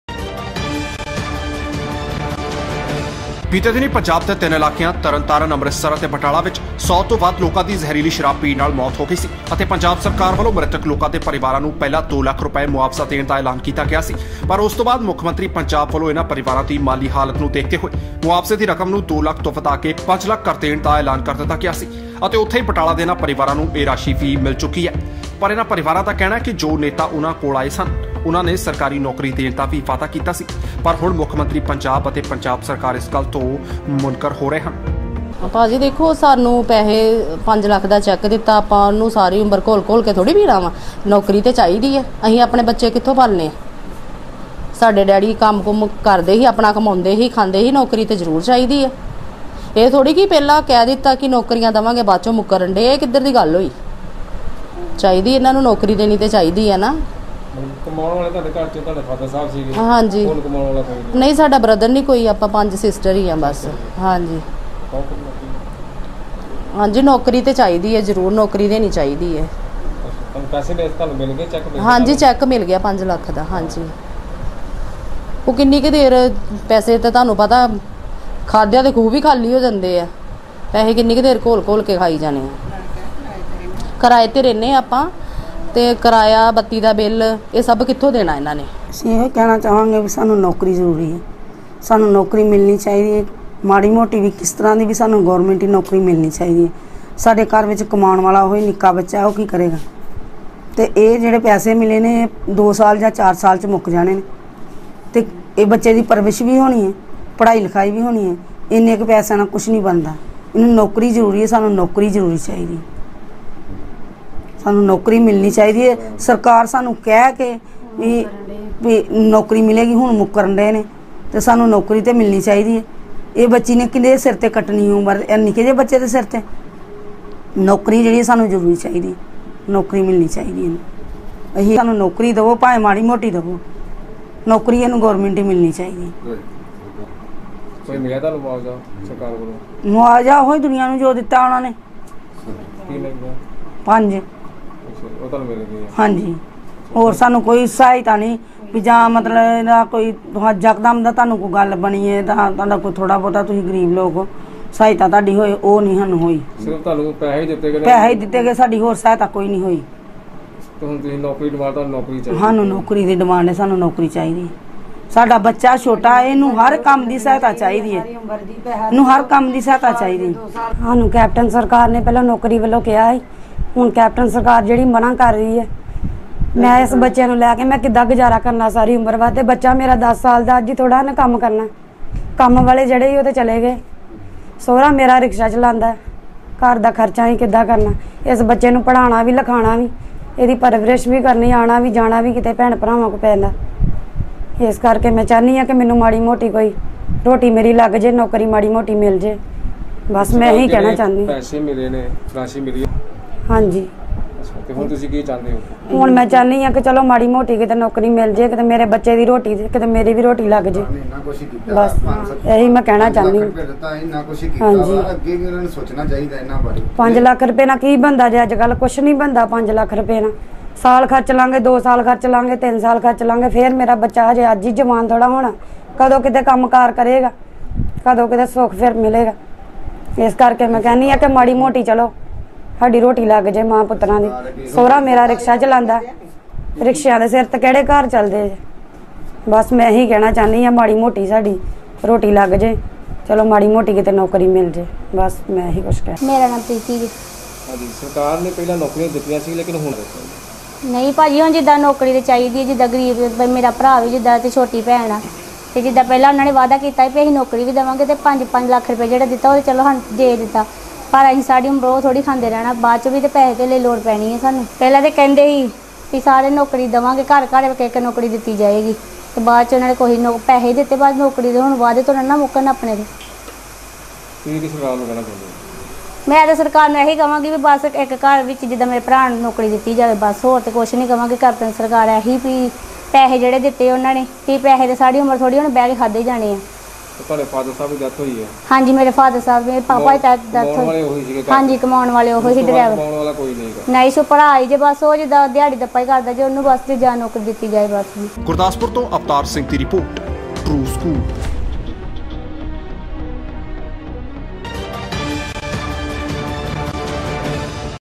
5 ਲੱਖ ਤਾਂ ਮਿਲੇ ਪਰ ਸਰਕਾਰੀ ਨੌਕਰੀ ਨਹੀਂ, ਜ਼ਹਿਰੀਲੇ ਜਾਮ ਨਾਲ ਮਰੇ ਲੋਕਾਂ ਦੇ ਪਰਿਵਾਰਾਂ ਦੀ ਮੰਗ
5 ਲੱਖ ਤਾਂ ਮਿਲੇ ਪਰ ਸਰਕਾਰੀ ਨੌਕਰੀ ਨਹੀਂ, ਜ਼ਹਿਰੀਲੇ ਜਾਮ ਨਾਲ ਮਰੇ ਲੋਕਾਂ ਦੇ ਪਰਿਵਾਰਾਂ ਦੀ ਮੰਗ